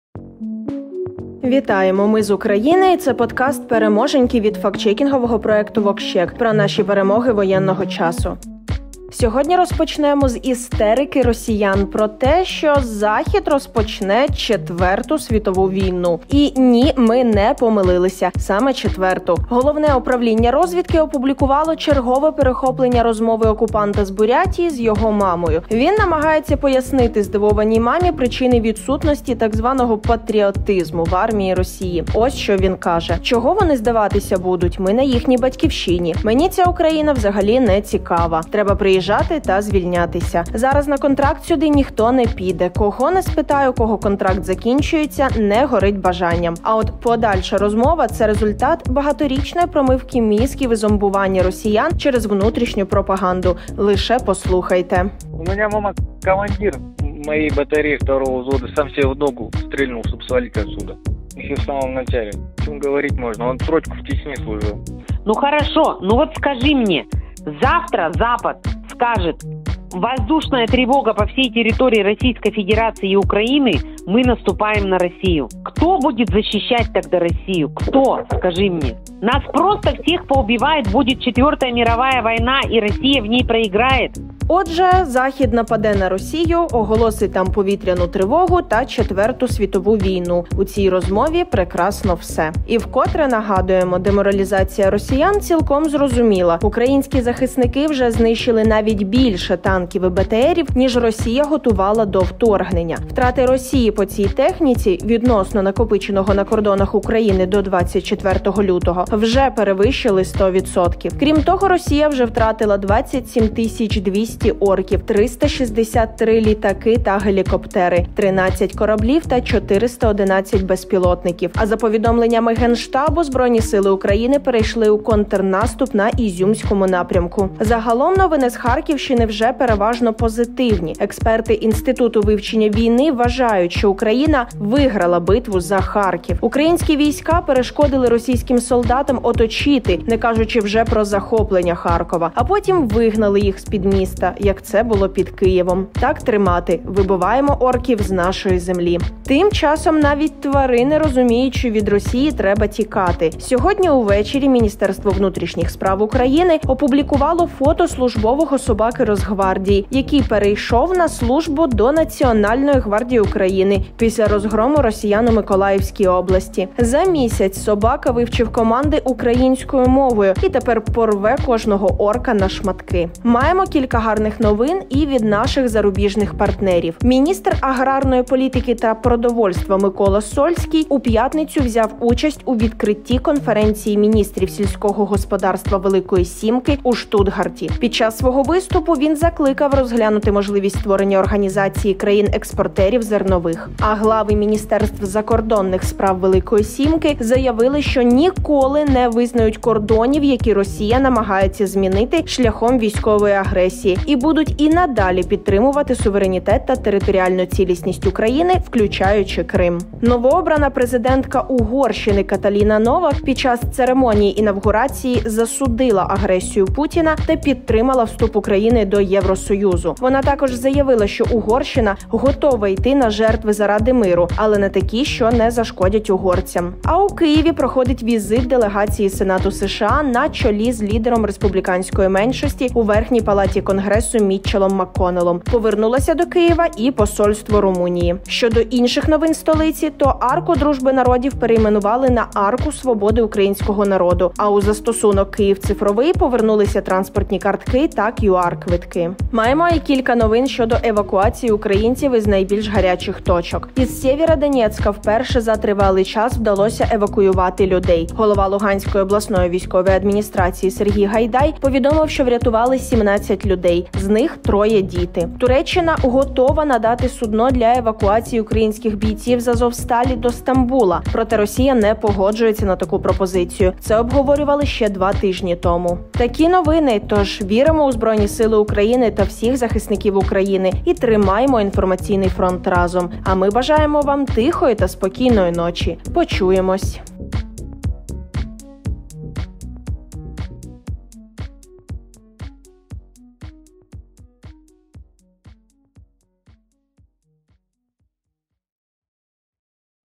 VoxCheck / подкаст Переможеньки 14/05
🔻Головне управління розвідки опублікувало чергове перехоплення розмови окупанта з бурятії з його мамою. Якщо коротко: Захід нападе на росію, оголосить там повітряну тривогу та четверту світову війну.

🔻Експерти Інституту вивчення війни вважаєть, що Україна виграла битву за Харків. Українські війська перешкодили російським солдатам оточити, не кажучи вже про захоплення Харкова, а потім вигнали їх з-під міста, як це було під Києвом.

🔻Новообрана президентка Угорщини Каталіна Новак під час церемонії інавгурації засудила агресію путіна та підтримала вступ України до Євросоюзу. Вона також заявила, що Угорщина готова «йти на жертви заради миру», але на такі, що не зашкодять угорцям.